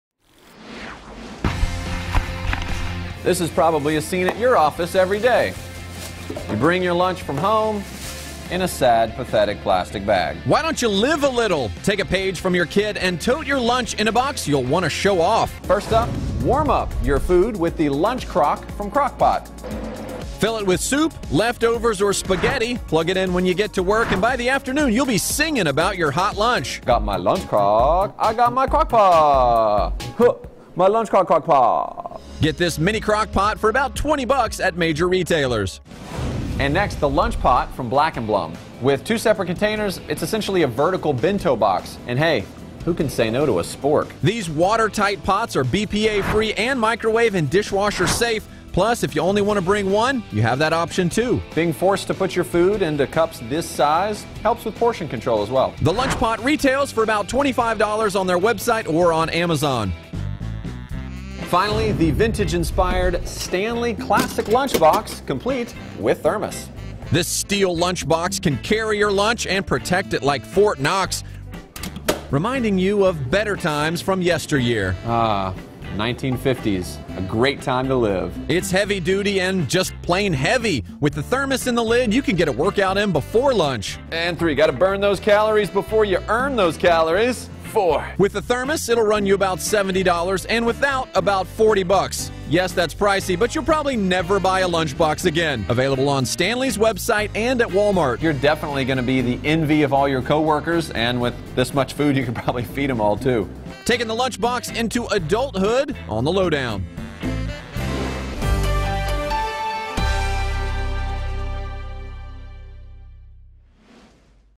3 Adult Lunch Boxes That Will Impress at Work
Are you tired of taking your food to work in a sad, pathetic plastic bag? Now you can be the most popular person in the breakroom! Bradley Hasemeyer has three cool lunch box options for adults.